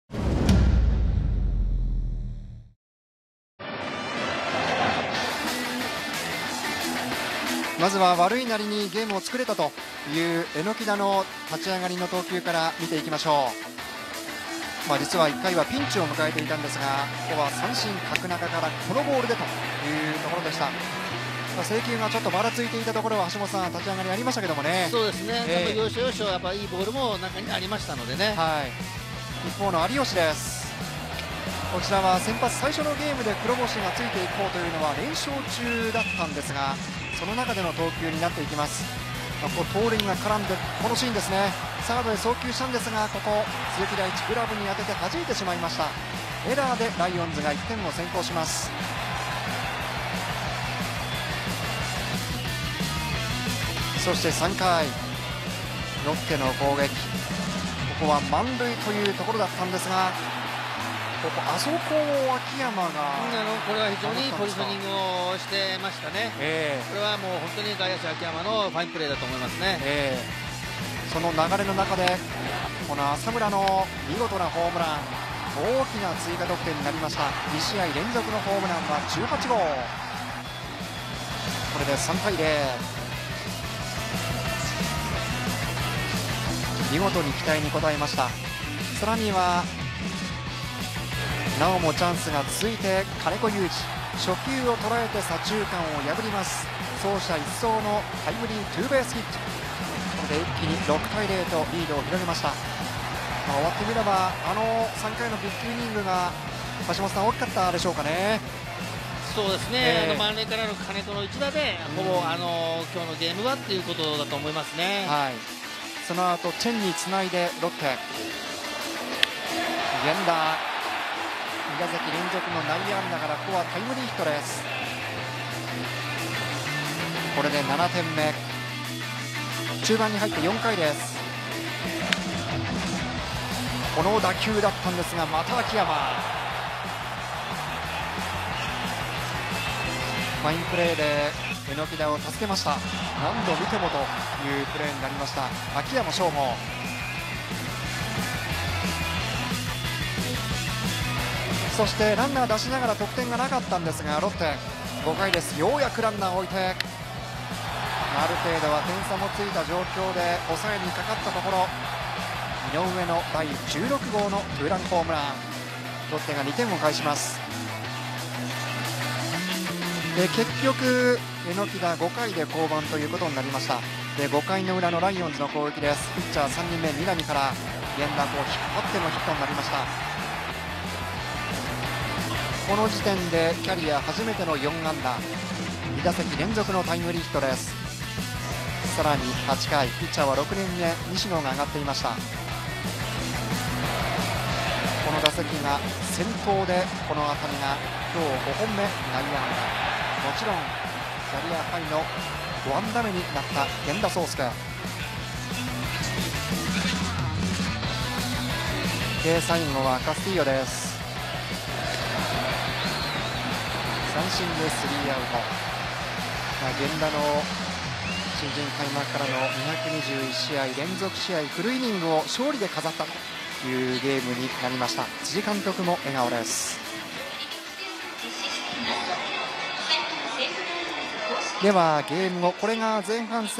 2018年7月11日 埼玉西武対千葉ロッテ 試合ダイジェスト
ライオンズ対マリーンズ(メットライフドーム)の試合ダイジェスト動画。 2018/07/11 埼玉西武ライオンズ 対 千葉ロッテマリーンズ